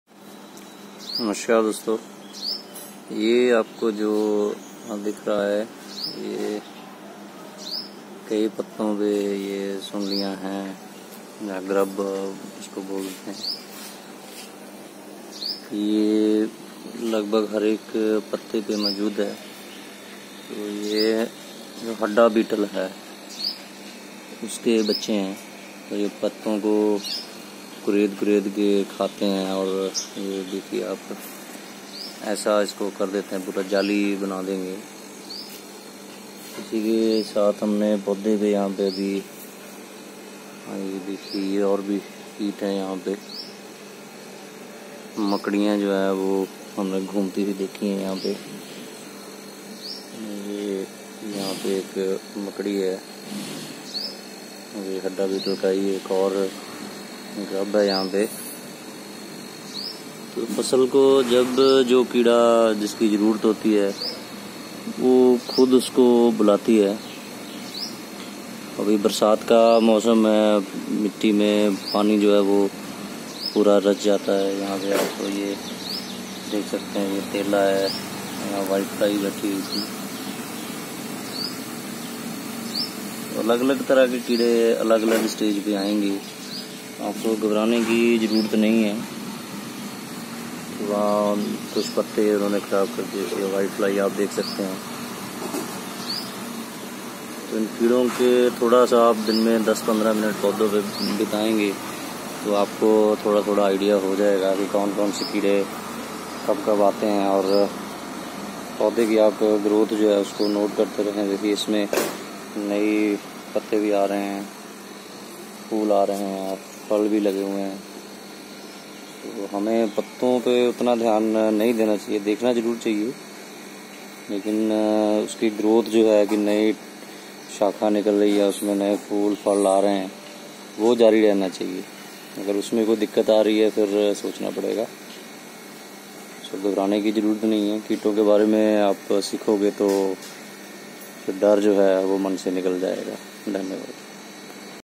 Pesticides Free Punjab Campaign - Hadda Beetle-2
Pesticides Free Punjab Campaign is run by Natural Farmers Association NFA in Punjab since 2017 under Dr. Surender Dalal Keet Saksharta Mission (Regd.) Jind Haryana.

All beetles are not carnivorous who eats or prey on other insects. Hadda beetle is a herbivorous insect which eats plant leaves by scratching them and sometimes scratches fruits also...